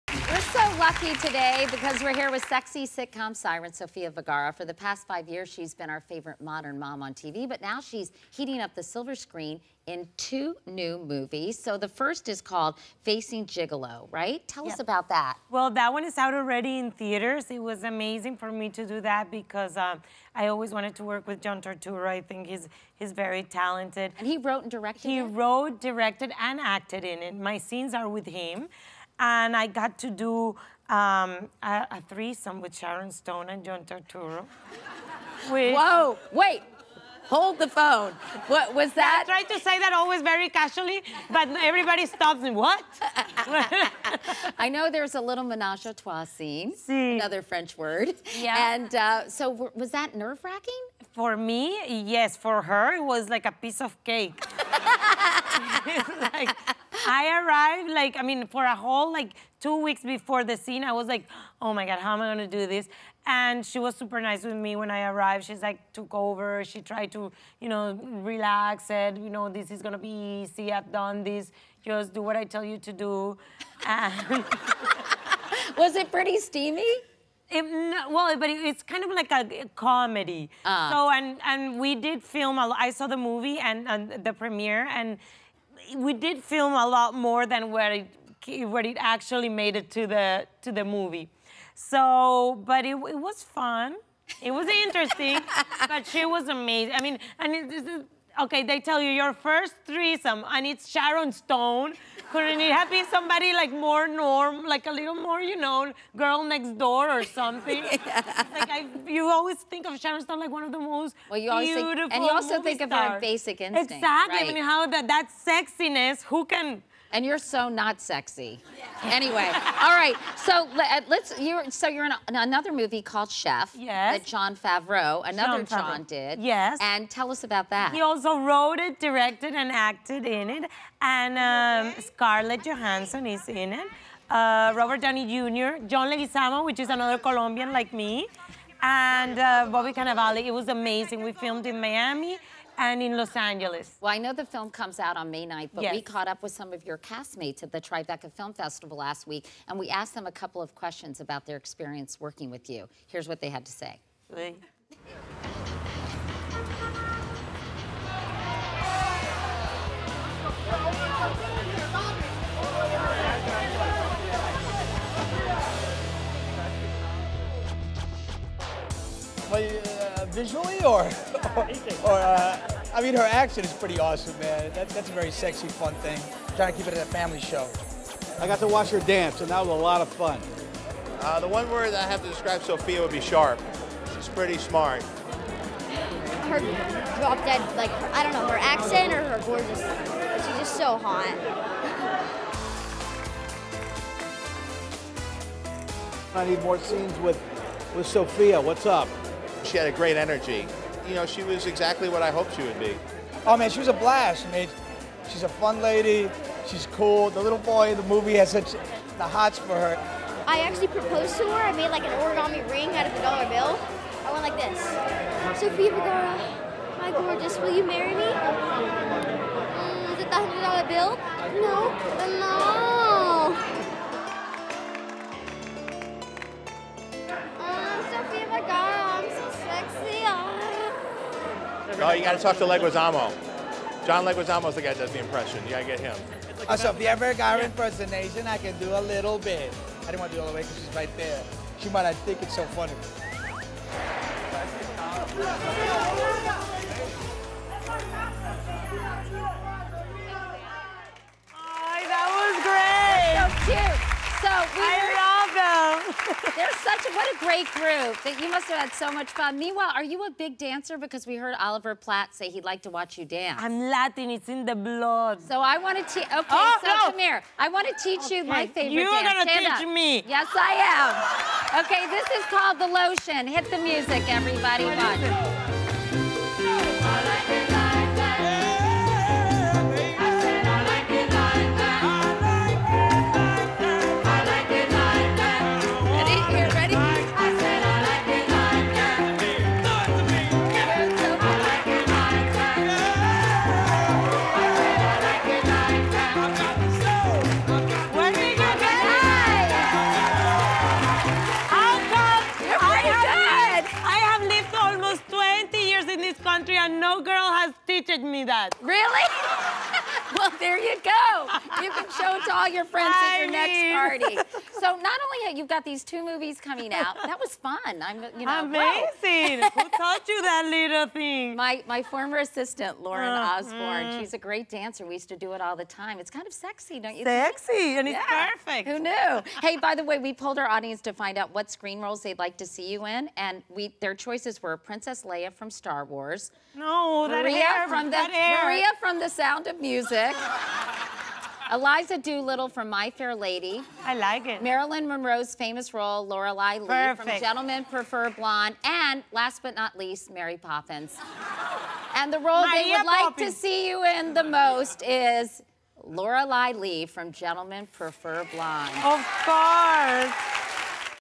What Sofia Vergara's Co-Stars Really Think About Her
For the past five years she's been our favorite "modern" mom on TV, but now she's heating up the silver screen in two new movies. We hit the red carpet to find out what Sofia's co-stars think about her. Plus, Katie shows Sofia some of her favorite dance moves!